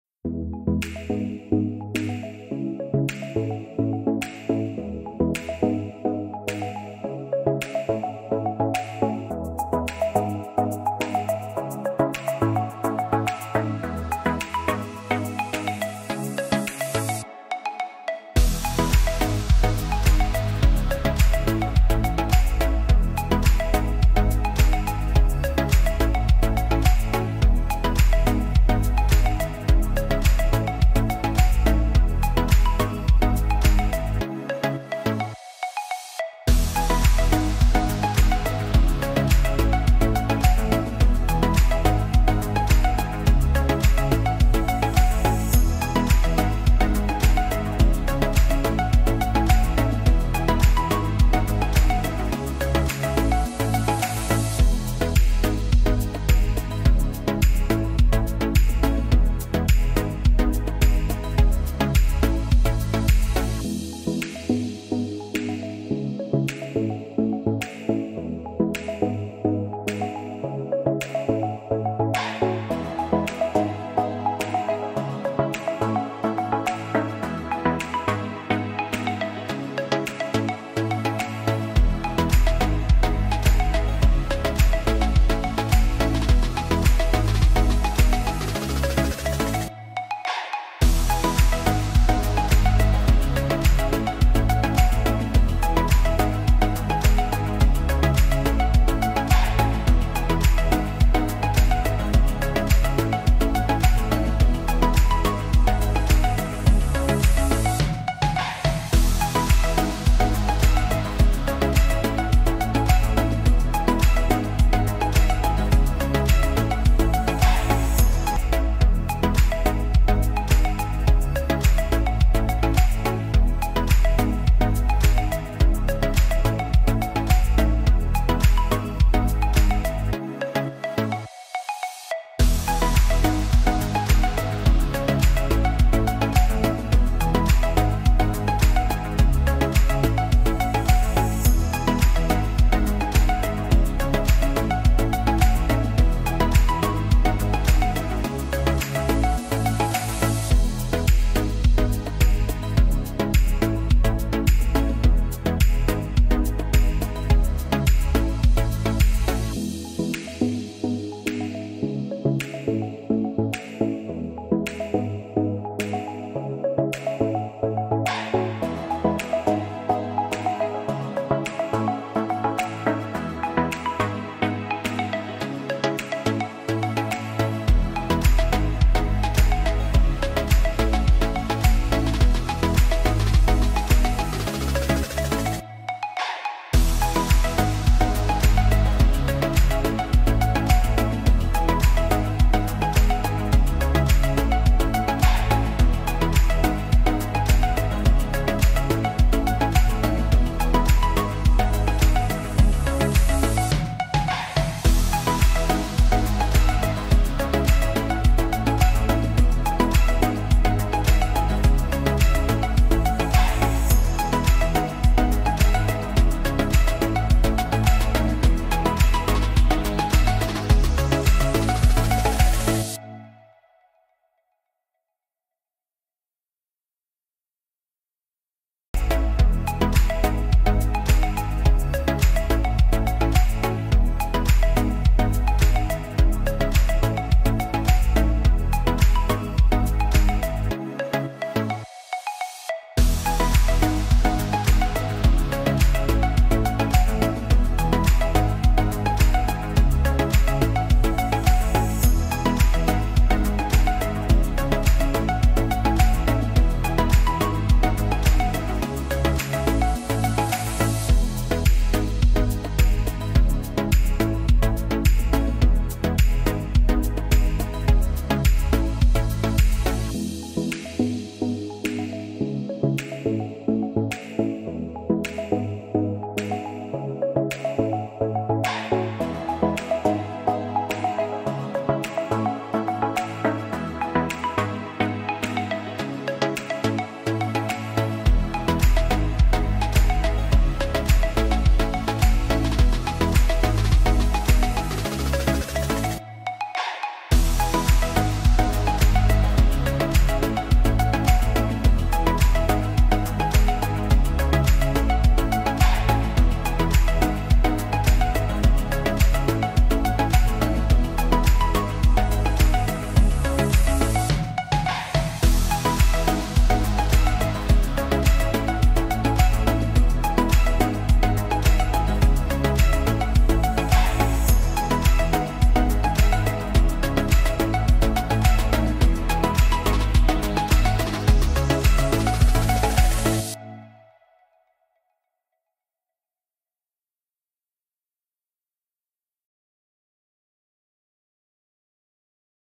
Costa Rica Mistico Arenal Hanging Bridges Tour | La Fortuna | 2023 Travel Vlog
Costa Rica's Mistico Arenal Hanging Bridges park is located at La Fortuna on the backdrop of Arenal Volcano mountain. It is home to thousands of unique species and has 16 hanging bridges. We were on a guided tour which consist of a 3 mile hike. Come join us as we explore the park on our adventurous journey !